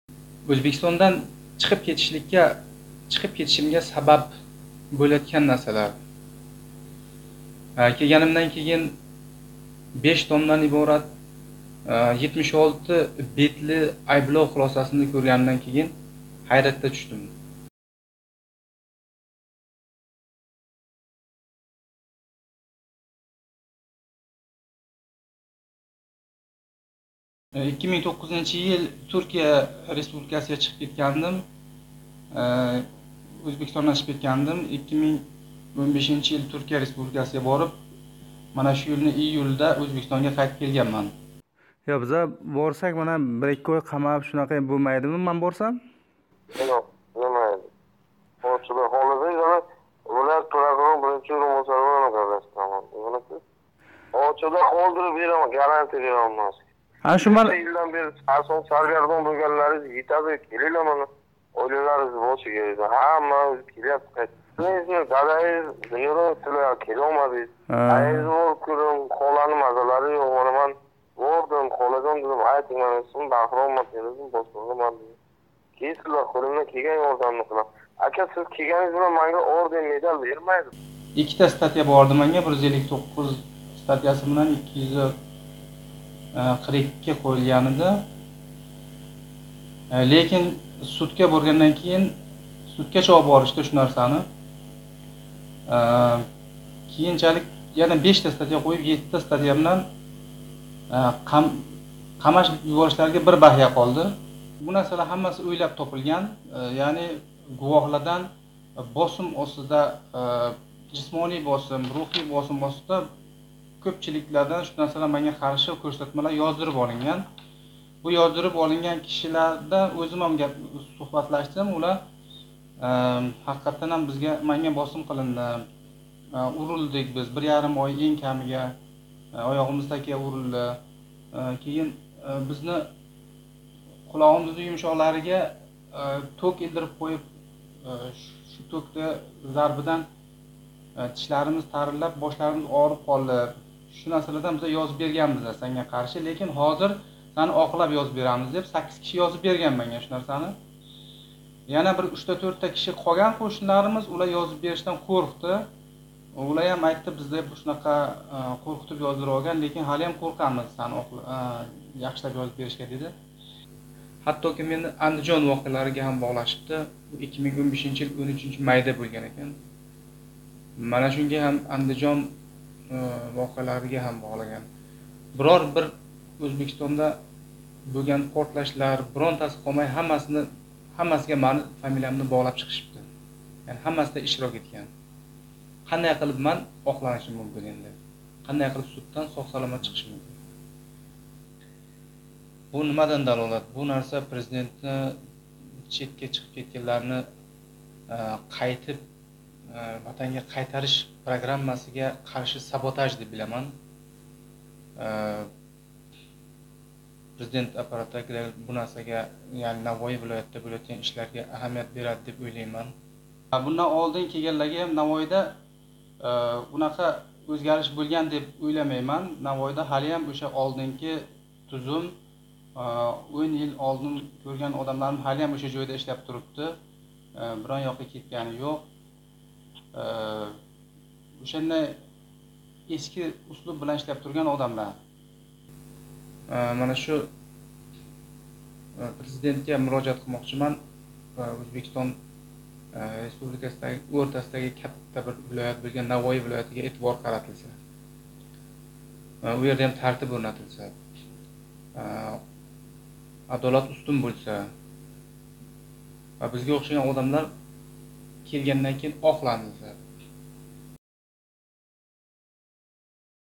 Ваъдаларга ишониб келган тадбиркор нега Ўзбекистондан қочди? - BBC Uzbek
Ички ишлар органларининг ваъдасига ишониб Туркиядан Навоийга қайтган тадбиркор Уйғун Саидов яна Ўзбекистонни ташлаб чиқишга мажбур бўлди. Унинг айтишича, Навоий вилоятига Мирзиёевнинг ислоҳотлари етиб бормаган.

2009 йили Россияга ишлашга кетган Уйғун Саидовга сиртдан конституциявий тузумга тажовуз қилиш ҳамда диний экстремистик гуруҳларни тузиш ва унда иштирок этиш айби қўйилиб жиноий иш очилади.

Тадбиркор қўйилган айбларнинг барчаси туҳматлиги ва бу иқрорларни ёзиб берганлар қийноқ остида шундай қилишга мажбур бўлишганини айтади.

Президент Шавкат Мирзиёев хорижда юрган ва жиноят содир этмаган фуқаролар Ўзбекистонга қайтиши мумкинлиги ва уларга нисбатан таъқиб бўлмаслигини айтганидан сўнг Туркияда оиласи билан яшаётган 47 ёшли Уйғун Саидовга Навоий вилоят орган ходимлари алоқага чиқади.

2018 йилнинг март ойида Уйғун Саидов ИИВнинг Навоий вилоят терроризм ва экстремизмга қарши курашиш бошқармаси бошлиғи Баҳром Қурбонов билан гаплашади.

Баҳром Қурбонов у оиласи билан Туркиядан қайтса унга нисбатан жиноий иш очилмаслиги ва уни яшил йўлак билан аэропортда кутиб олишларини ваъда беради. Баҳром Қурбонов бу борада Навоий вилоят прокурорининг биринчи ўринбосари билан ҳам гаплашиши мумкинлигигини айтади.

Бироқ шу йилнинг июль ойида Ўзбекистонга қайтиб келган Уйғун Саидовга яна 7 та жиноий иш очилади ва иши зудлик билан судга оширилади.

Бу вақтда унга нисбатан қамоққа олиш эҳтиёт чораси 30 миллион сўмлик гаров пули эвазига уй қамоғига алмаштирилади.

Тадбиркор ўзини алданган ҳис қилган ва унинг таъкидлашича, иш уни бир неча йил қамоққа тиқиш билан якунланишига қараб кетаётганидан сўнг Ўзбекистонни ташлаб қочишга мажбур бўлади.

Ҳозир унга нисбатан қидирув эълон қилинган.

"Мени Ўзбекистонда нечта портлаш бўлган бўлса ҳаммасида айблаб ташлашибди. Ҳатто 2005 йил май ойида Андижонда бўлган отишмаларда ҳам айблашибди. Менга қарши гувоҳлик берган одамлар билан гаплашдим. Уларнинг айтишича, оёқларининг остига дубинка билан уриб, қулоқларига сим осиб ток урдиргандан кейин мажбуран менга қарши кўрсатма ёзиб беришган экан", дейди Уйғун Саидов.

Тадбиркор ўзига қарши гувоҳлик берган саккиз кишидан кўрсатмасини ўзгартиргани ҳақида ариза олиб борган. Бироқ терговчи ушбу кўрсатмаларга ҳам қарамасдан унга нисбатан жиноий иш очган.

"Мирзиёевга мурожаат қилмоқчиман. Ўзбекистоннинг ўртасида жойлашган Навоий вилоятида ҳам тартиб ўрнатсин. Порталга ёзган шикоятларимиз ҳаммаси ўзимизга қайтиб боряпти. Ҳеч қандай чора кўрилмаяпти", дейди Ўзбекистонни ташлаб чиқишга мажбур бўлган Уйғун Саидов.